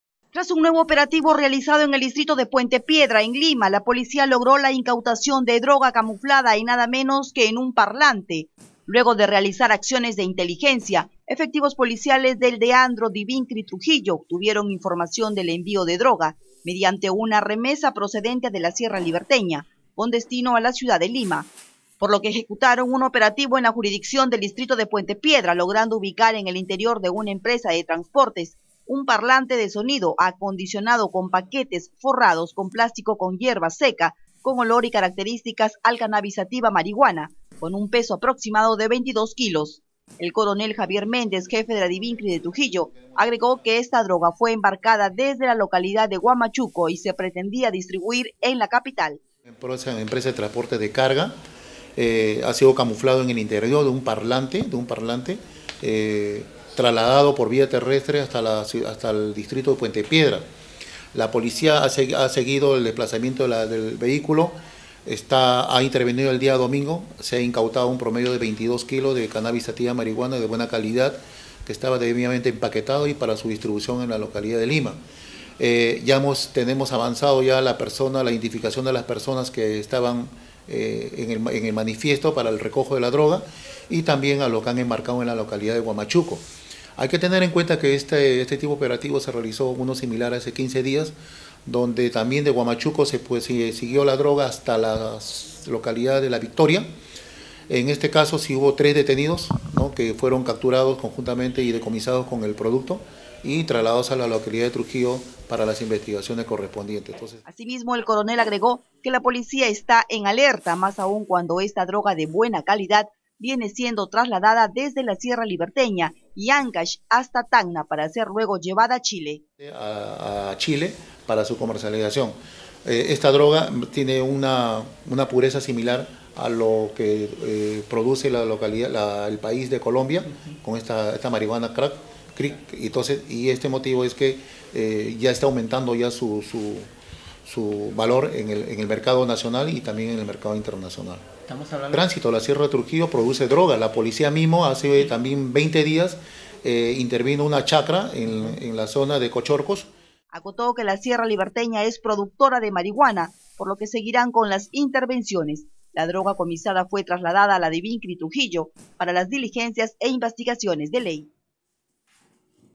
PNP incauta droga camuflada en un parlante
Durante operativo la Policía Nacional decomisó 20 kilos de marihuana en el distrito de Puente Piedra. Esto luego de realizar acciones de inteligencia. Fue la DIVINCRI Trujillo quien obtuvo información del envió de droga mediante una remesa procedente de la sierra liberteña.

Tras un nuevo operativo realizado en el distrito de puente piedra en lima, la policía logró la incautación de droga camuflada en nada menos que en un parlante.

Luego de realizar acciones de inteligencia, efectivos policiales del AREANDRO – DIVINCRI Trujillo obtuvieron información del envío de droga, mediante una remesa procedente de la sierra liberteña, con destino a la ciudad de lima, por lo que ejecutaron un operativo en la jurisdicción del distrito de Puente Piedra logrando ubicar en el interior de una empresa de transportes, un parlante de sonido acondicionado con paquetes forrados con plástico con hierba seca con olor y características al Cannabis sativa (marihuana), con un peso aproximado de 22 kilos.

El coronel Javier Méndez, jefe de la DIVINCRI de Trujillo, agregó que esta droga fue embarcada desde la localidad de Huamachuco y se pretendía distribuir en la capital.

Asimismo, el coronel agregó que la policía está en alerta, más aún cuando esta droga de buena calidad viene siendo trasladada desde la sierra liberteña y Áncash hasta Tacna para ser luego llevada a Chile.

Acotó que la sierra liberteña es productora de marihuana por lo que seguirán con las intervenciones. La droga comisada fue trasladada a la DIVINCRI Trujillo, para las diligencias e investigaciones de ley.